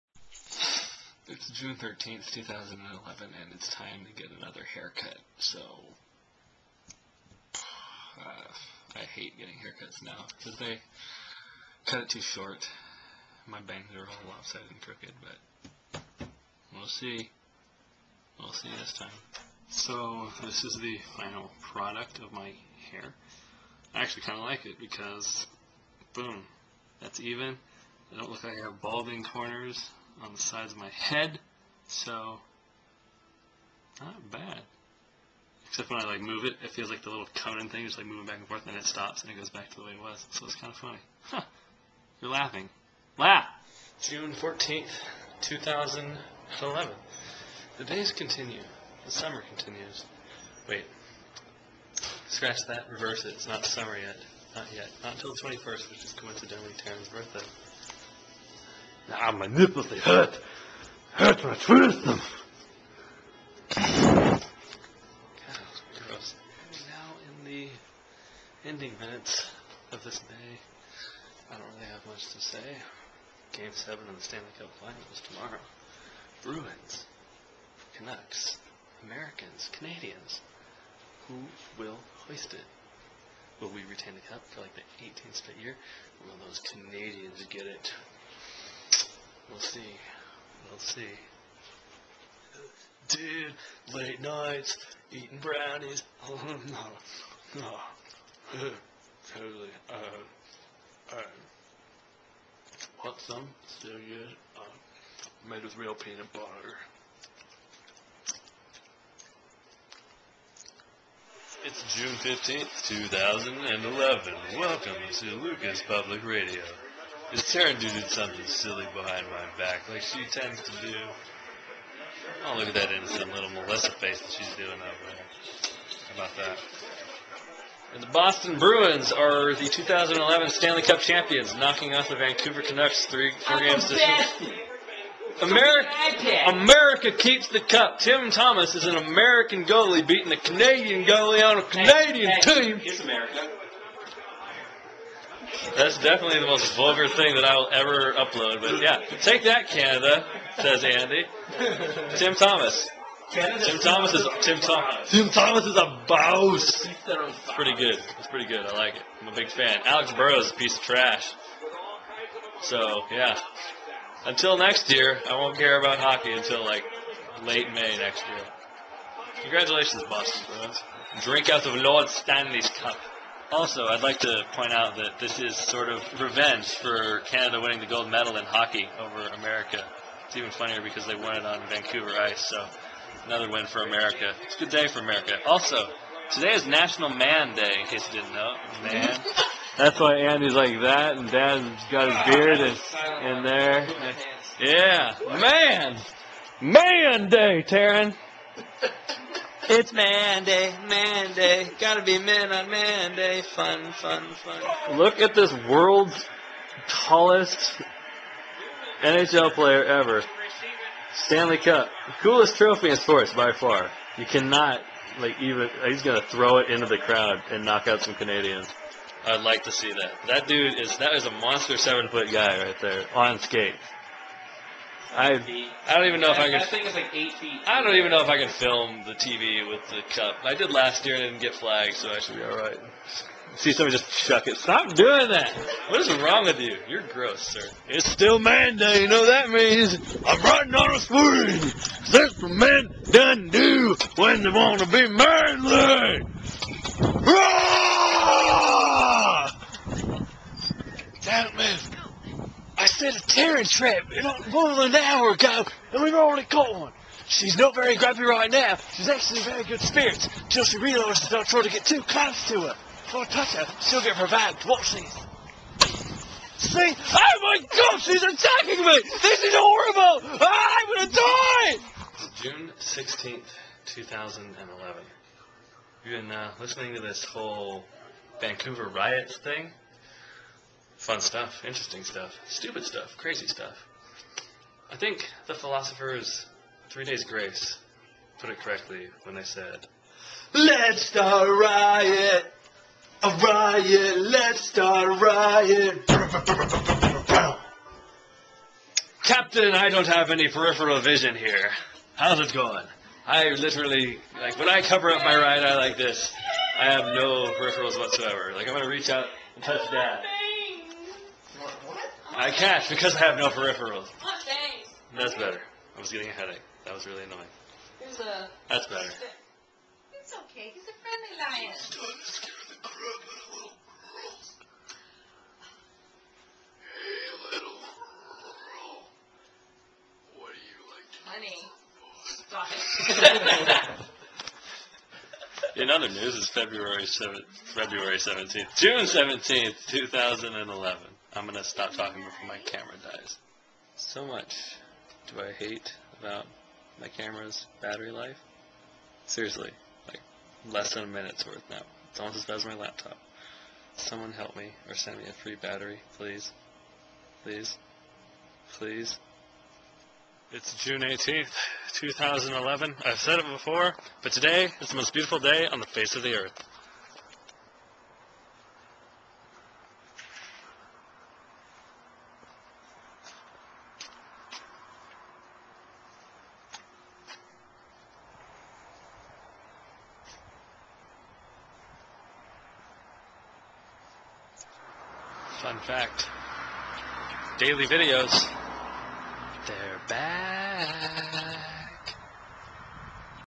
Week 24 of 2011 - Haircut, Boston Bruins, Vancouver Riots
Hey weirdos, more haircut stuff! First time in awhile I haven't had a horrible one! In other news, the Boston Bruins won the Stanley Cup over the Vancouver Canucks. Speaking of Vancouver...riots.